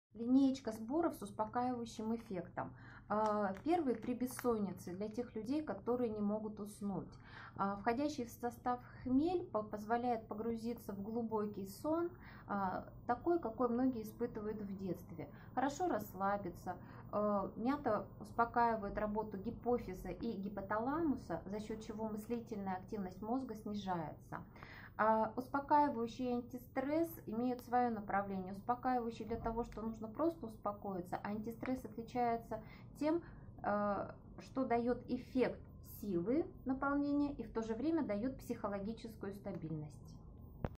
Алтайские успокаивающие сборы
Уральская травница Казанчеева Татьяна рассказывает о алтайских успокаивающих сборах "Антистресс", "При бессонице", "Успокаивающий".

Приобрести сборы можно в Челябинске, в магазине "Травяная лавка" по адресу Плеханова, 45.

С информацией о травах, рецептах и траволечении вы можете познакомиться на канале уральской травницы Татьяны Казанчеевой